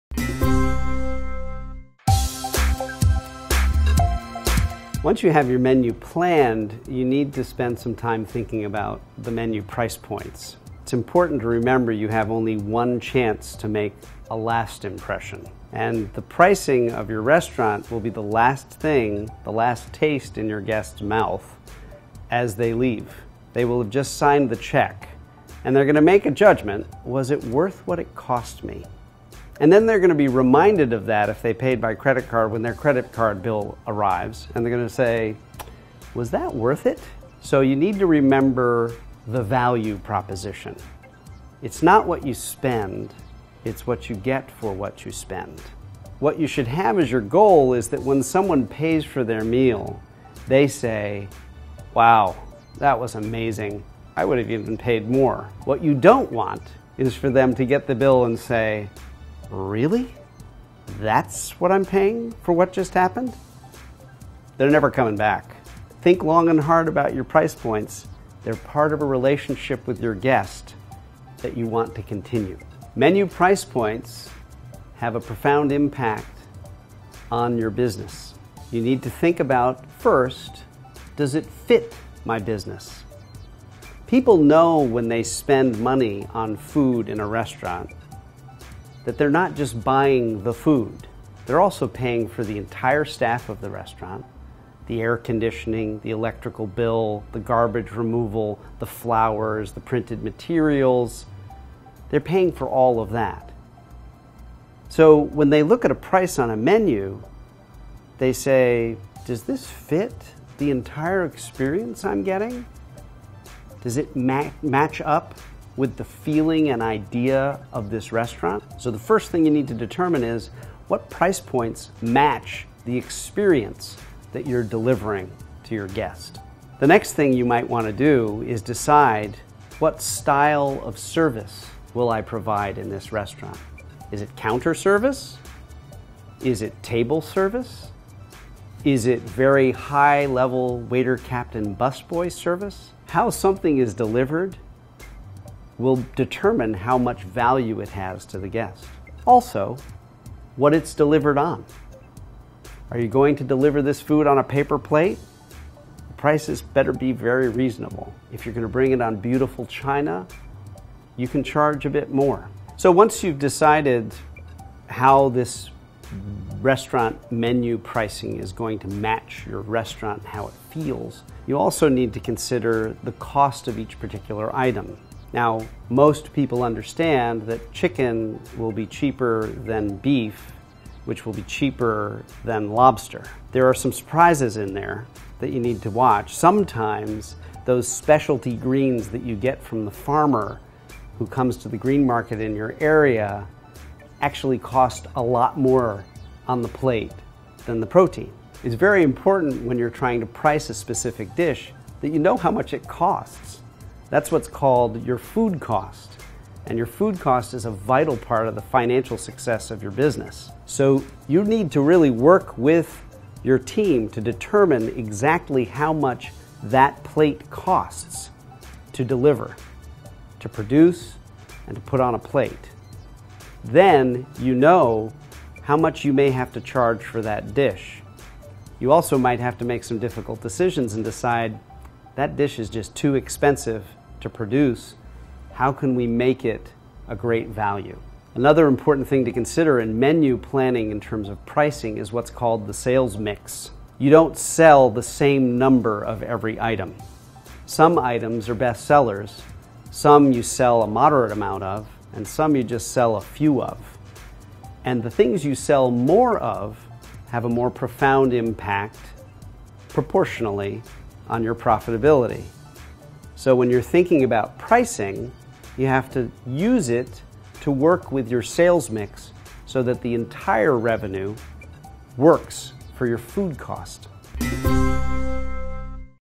How to Determine Menu Price Point | Restaurant Business
Once you have your menu planned, you need to spend some time thinking about the menu price points.  It's important to remember that you have only one chance to make a last impression and the pricing of your restaurant will be the last thing, the last taste, in your guests' mouth as they leave.

They will have just signed the check and they're going to make a judgment, "Was it worth what it cost me?"  And then they're going to be reminded of that, if they paid by credit card, when their credit card bill arrives and they're going to say "Was that worth it?"

So you need to remember the value proposition.  It's not what you spend, it's what you get for what you spend.

What you should have as your goal is that when someone pays for their meal, they say "Wow!  That was amazing.  I would have even paid more."

What you don't want is for them to get the bill and say "Really?  That's what I'm paying for what just happened?"  They're never coming back.

Think long and hard about your price points; they are part of a relationship with your guests that you want to continue.

Menu price points have a profound impact on your business.  You need to think about first, does it fit my business?  People know when they spend money on food in a restaurant, that they're not just buying the food.  They're also paying for the entire staff of the restaurant, the air conditioning, the electrical bill, the garbage removal, the flowers, the printed materials; they're paying for all of that.

When they look at a price on a menu, they say "Does this fit the entire experience I'm getting?  Does it match up with the feeling and idea of this restaurant?"

So the first thing you need to determine is "What price points match the experience that you're delivering to your guests?"

The next thing that you might want to do is decide "What style of service will I provide in this restaurant?"  Is it counter service?  Is it table service?  Is it very high-level waiter/captain/bus boy service?  How something is delivered will determine how much value it has to the guests.

Also, what it's delivered on.  Are you going to deliver this food on a paper plate?  Prices better be very reasonable.  If you're going to bring it on beautiful china, you can charge a bit more.

Once you've decided how this restaurant menu pricing is going to match your restaurant and how it feels, you also need to consider the cost of each particular item.  Now most people understand that chicken will be cheaper than beef, which will be cheaper than lobster; there are some surprises in there that you need to watch.

Sometimes those specialty greens that you get from the farmer, who comes to the green market in your area, actually cost a lot more on the plate than the protein.

It is very important when you're trying to price a specific dish that you know how much it costs; that's what is called your food cost and your food cost is a vital part of the financial success of your business.

So you need to really work with your team to determine exactly how much that plate costs to deliver, to produce, and to put on a plate.  Then you know how much you may have to charge for that dish.

You also might have to make some difficult decisions and decide that dish is just too expensive to produce.  How can we make it a great value?

Another important thing to consider in menu planning, in terms of pricing, is what's called the sales mix.  You don't sell the same number of every item.  Some items are best-sellers, some you sell a moderate amount of, and some you just sell a few of.

The things you sell more of, have a more profound impact, proportionally, on your profitability.  So when you're thinking about pricing, you have to use it to work with your sales mix so that the entire revenue works for your food costs.